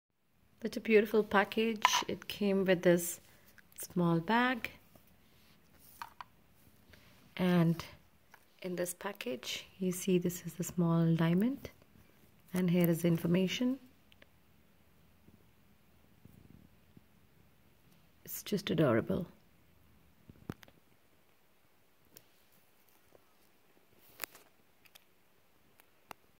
Glitz design 0.01 c.f. round brilliant cut 1.30 'll
Glitz Design 0.01 ct Round Brilliant Cut 1.30 mm J I1 Loose Diamond Natural Earth-mined
This is a small, pretty diamond, it is quite tiny so easy to loose.
The diamond has Clarity I1-I2.
The diamond is Conflict free Diamonds Natural, Genuine & Earth-mined
It came beautifully packaged and can be a great gift idea as well.